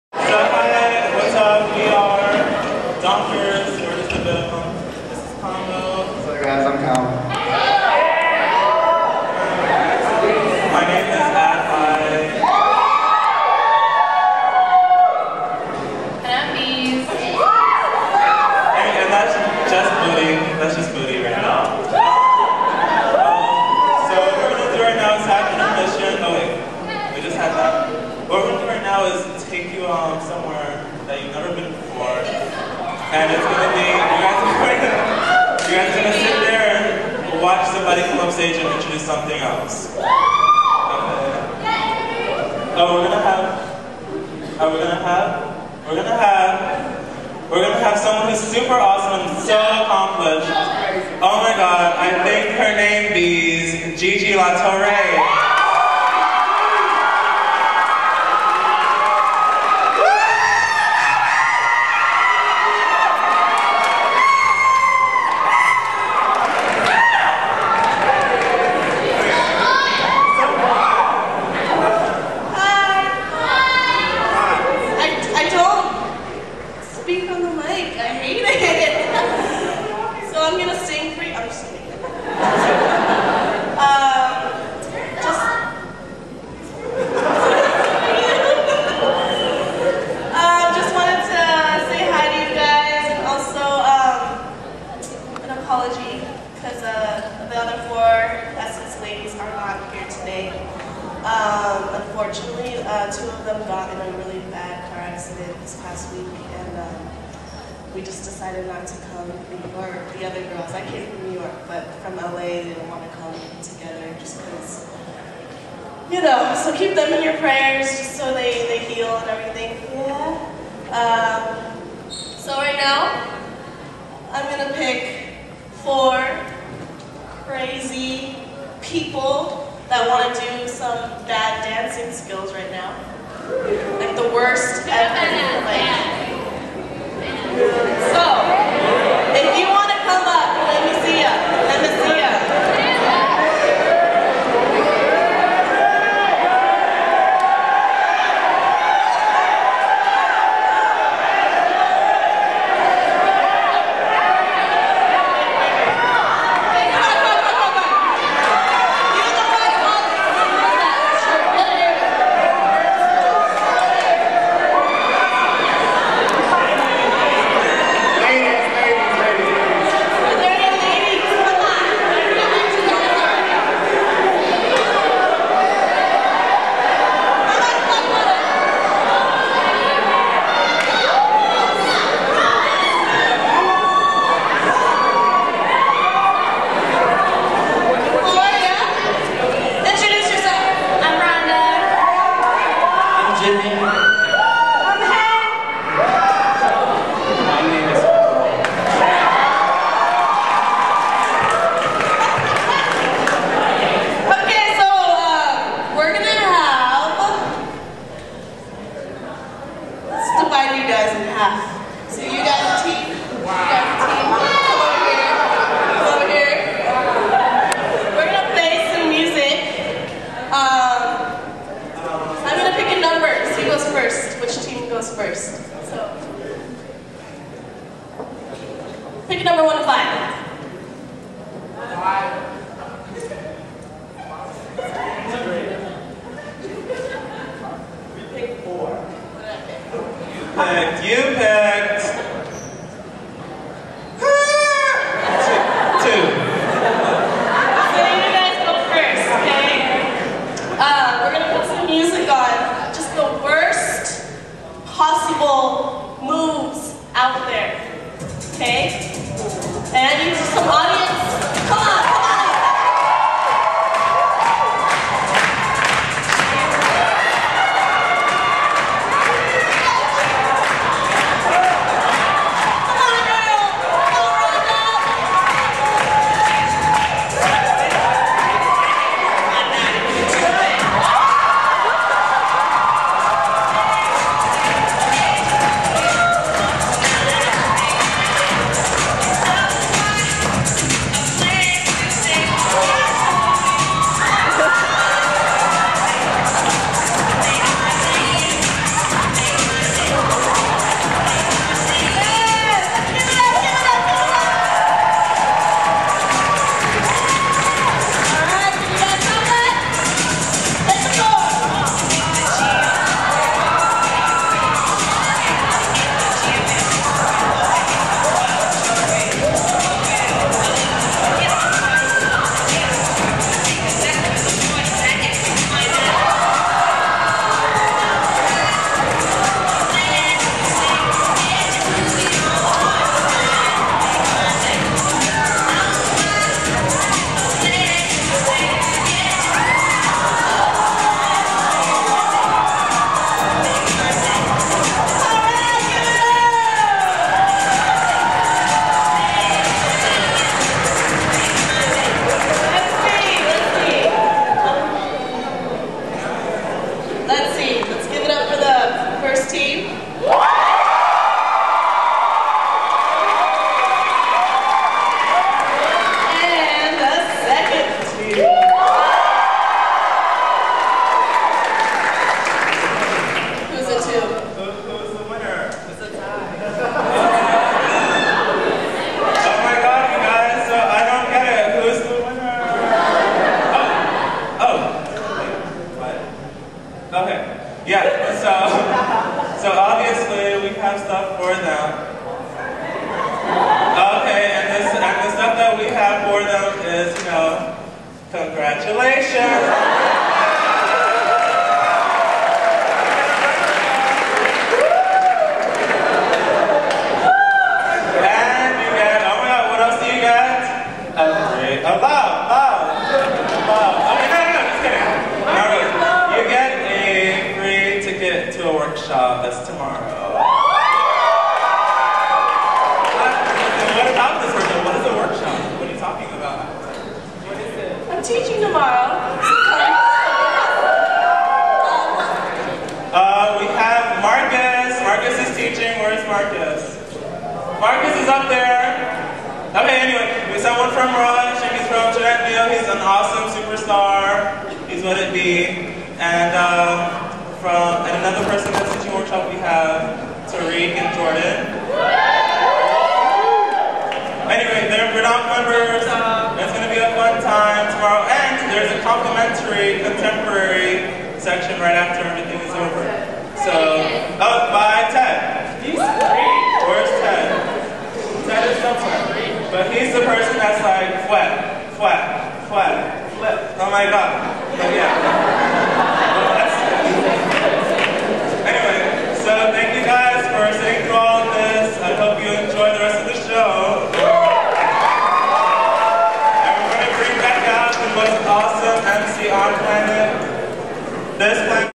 Footwork 2009: Bad Dance Skillz Contest hosted by Gigi Torres
The lovely Gigi Torres and Voluptuous Adlai Grayson host the Bad Dance Skillz Contest at Footwork 2009!